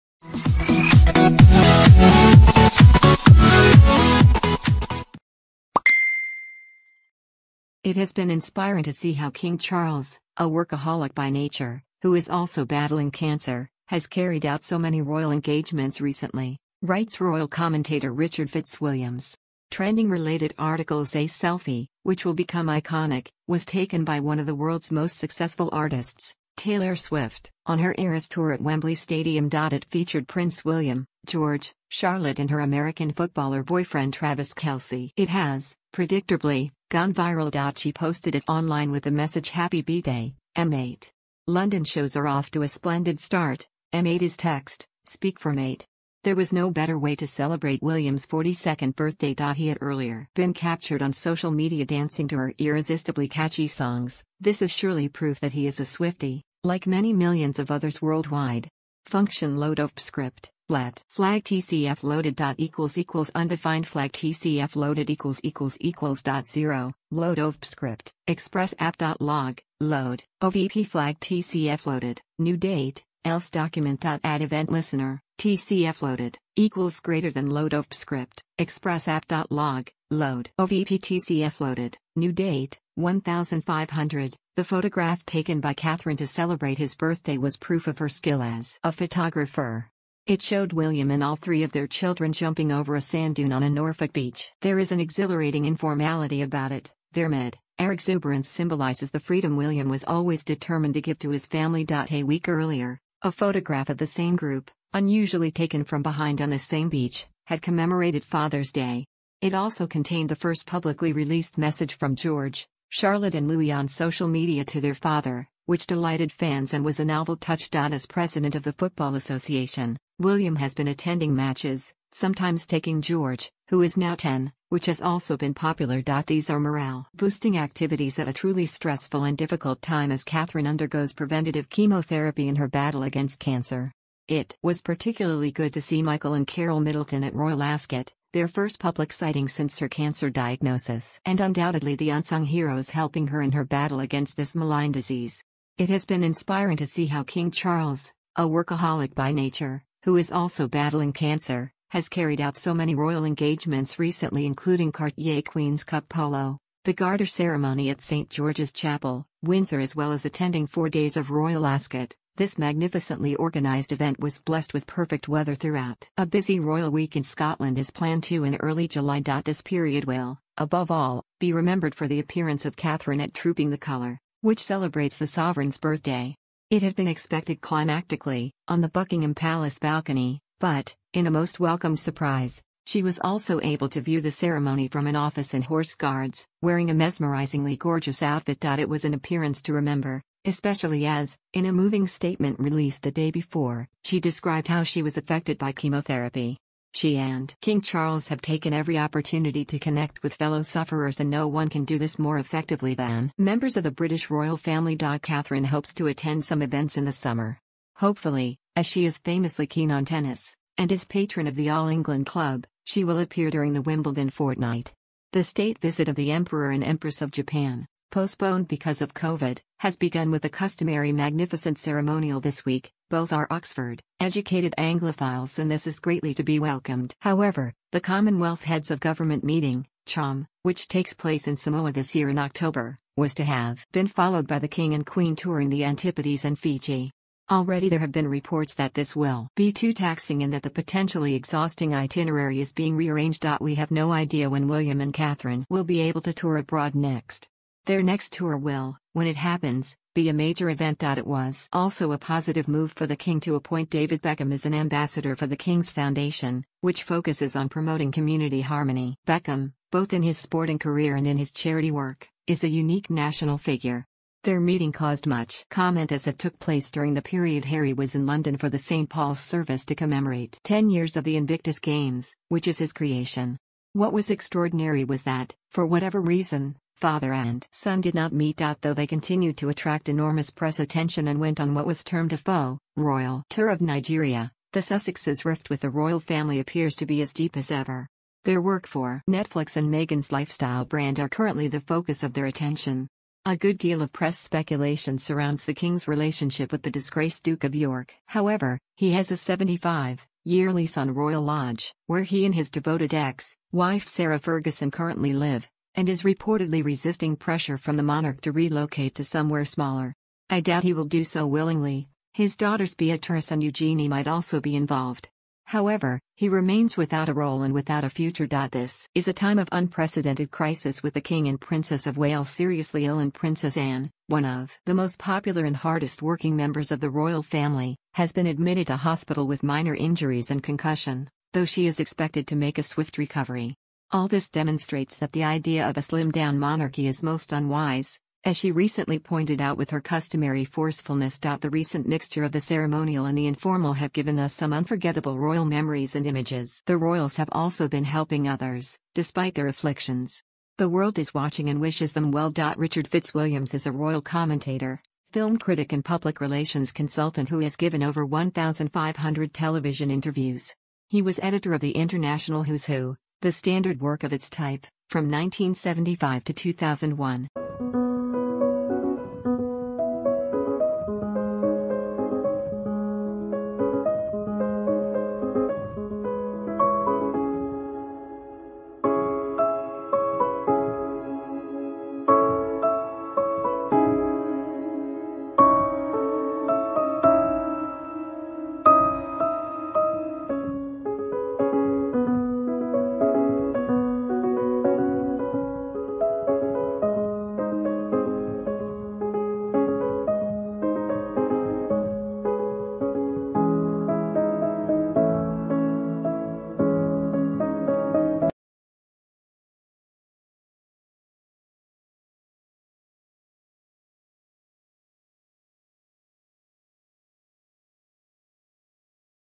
Royal Family News Latest: Royal Family in 'unprecedented crisis' and King Charles must avoid ma...
Royal Family News Latest: Royal Family in 'unprecedented crisis' and King Charles must avoid making 'unwise' move. royal family news latest It has been inspiring to see how King Charles, a workaholic by nature, who is also battling cancer, has carried out so many royal engagements recently, writes royal commentator Richard Fitzwilliams. Trending Related articles A selfie, which will become iconic, was taken by... royal family news latest